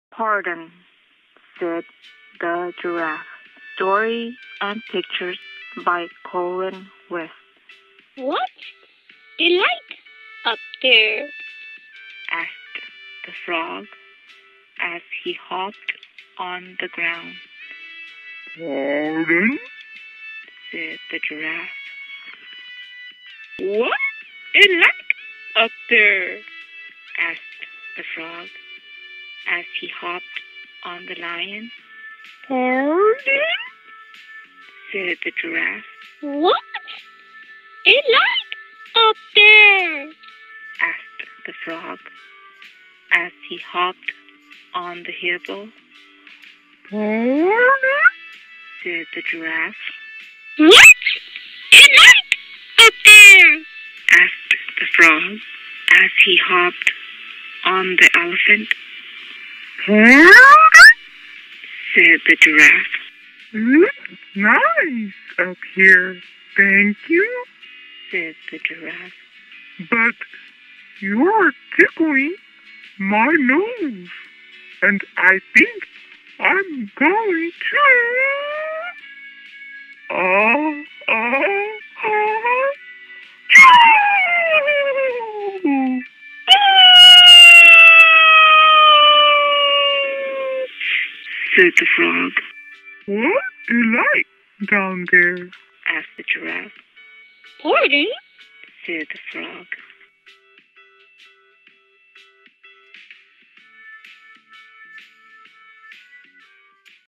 "PARDON?" SAID THE GIRAFFE- children's book about curiosity
Frog wants to know how it's like to be up high, so when he saw giraffe he decided to ask. What does curiosity leaves him with. Author of this book is Colin West. Watch to see what happens...